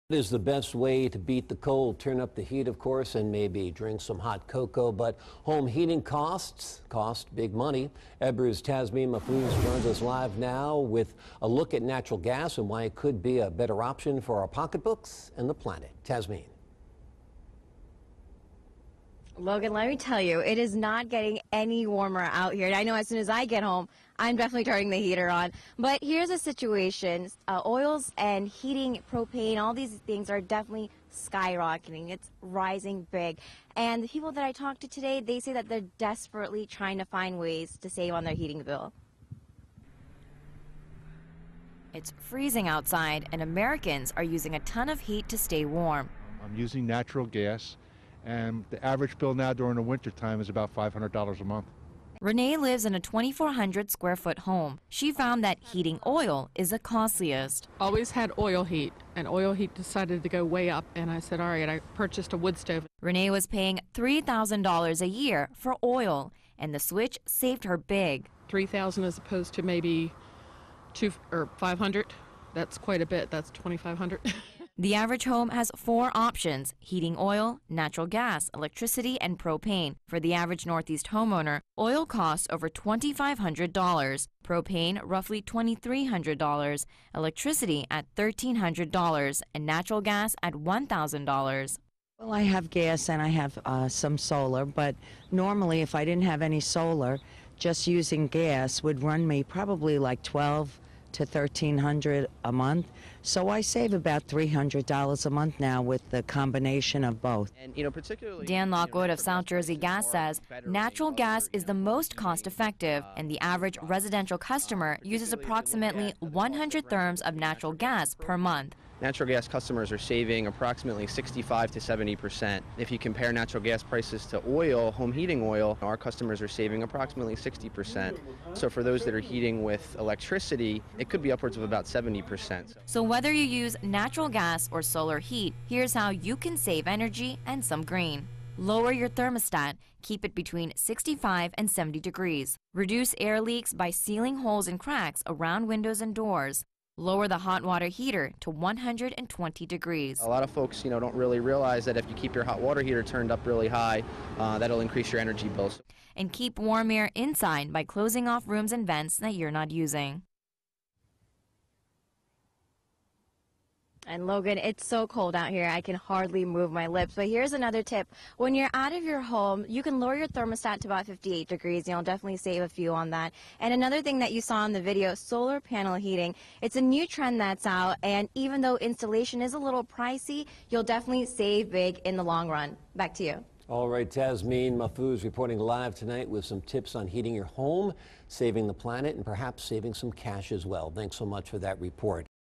How Much Does Home Heating Cost?
What's the best way to beat the cold? Turn up the heat. But home heating costs big money. Ebru's Tasmin Mahfuz tells  why natural gas is a better option for your pocket and the planet.